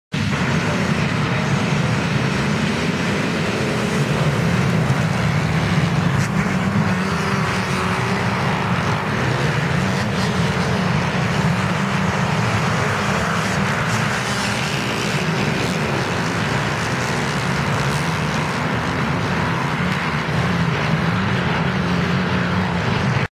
Buddscreek Marissa Peck, Moto 2 (2/27/2011)
Marissa Peck Start Moto 2, #229 monster gear, womens class split with schoolboy